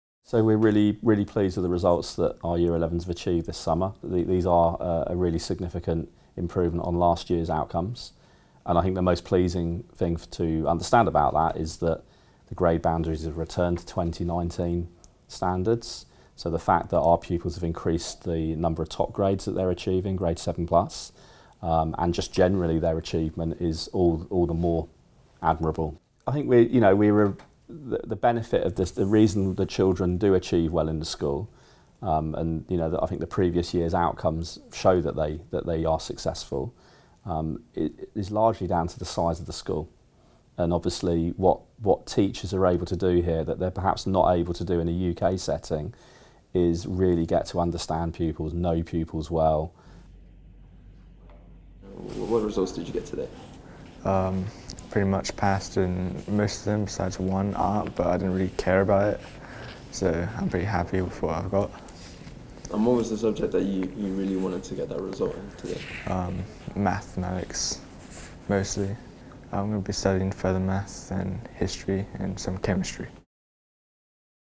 The Falkland Islands students are receiving their GCSE results
For the Year 11 Students of the Falkland Islands Community School, August Twenty fourth has most likely been a date circled in most of their calendars for some time; with it being the release of their GCSE results.
With reported, “Exceptional rises in English and Maths grades” as well as the indication that “the outcomes of pupils at FICS in 2023 show a rise on 2022 achievements”.
We went along to find out more.